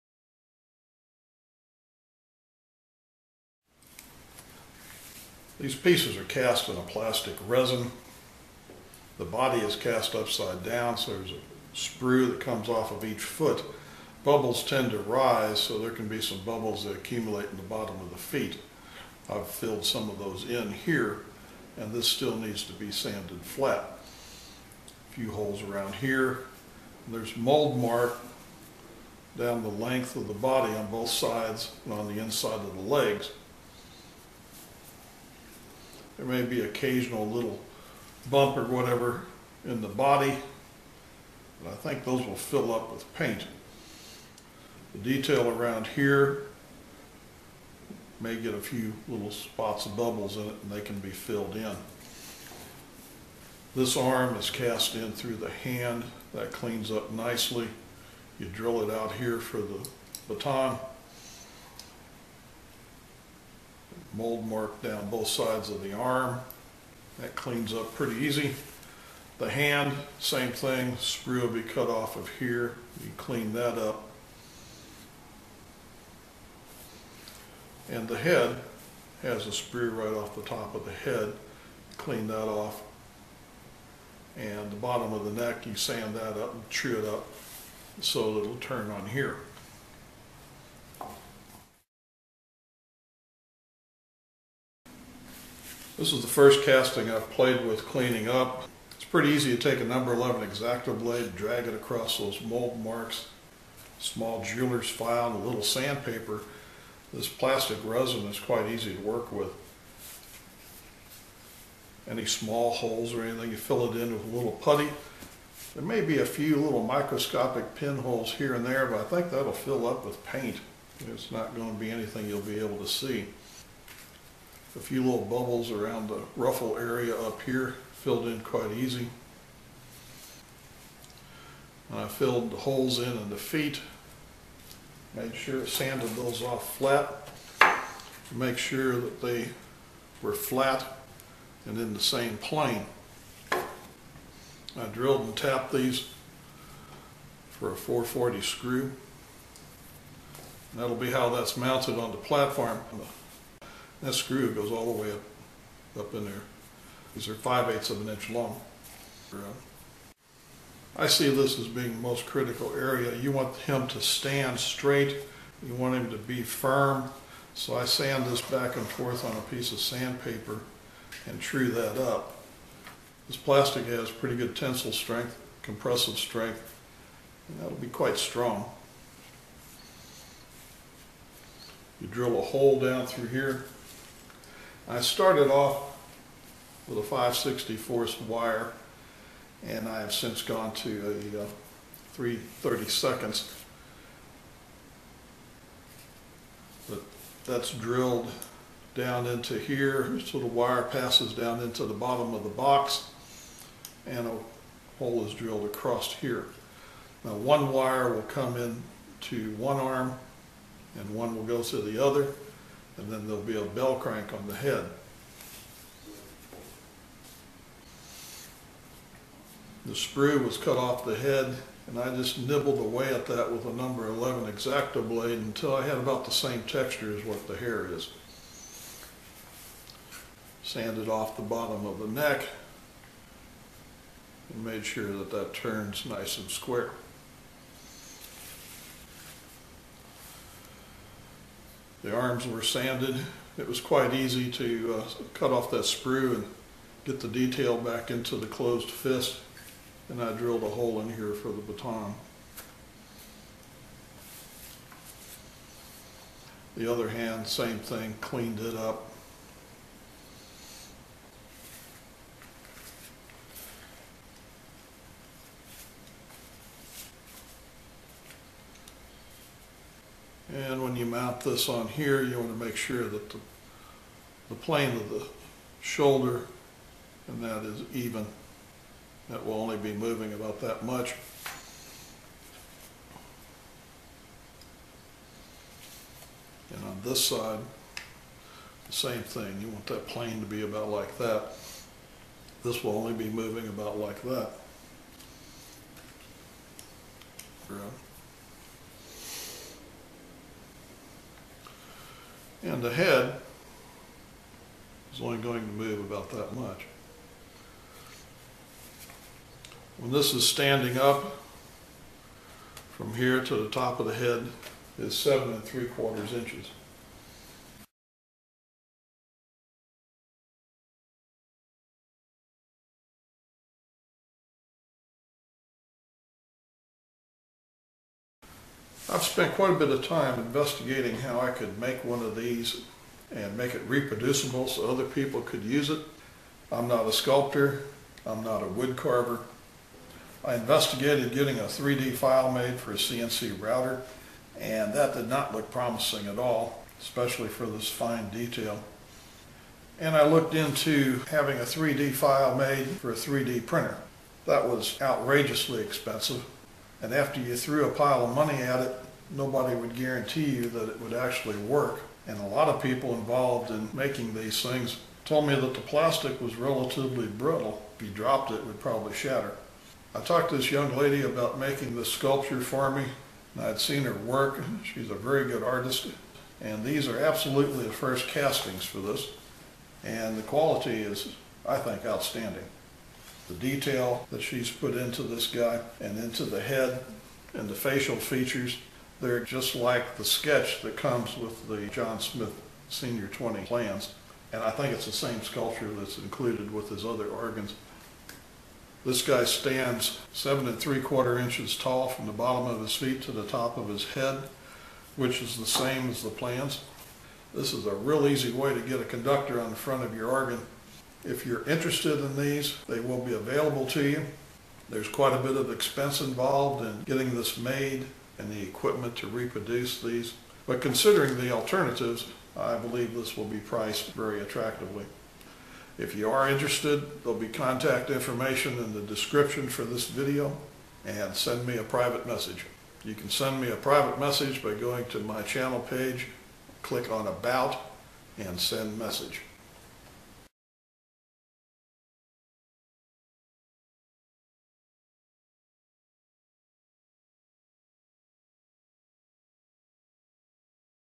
Senior 20 Organ Conductor (1) Overview
A review of the very first rough castings for the prototype conductor figure. Several changes have since been made to the figure to improve its performance. Your castings will be from the improved figure.

This project started over 18 months ago. The objective was a high quality, finely detailed, reproducible, animated, conductor figure suitable for use on the front of John Smith (or similar) organs. I am not a wood carver, nor a sculptor. I can only assume others suffer similar limitations and this is why few organs include a conductor.

The figure was first sculpted with great detail, and then a series of molds were made for casting the figure in resin.

A high quality castings kit will be available for a limited time.

I will not respond to this in the comments section of the videos.  If you are interested in purchasing a Conductor Castings Kit, please go to my channel page. Please be patient.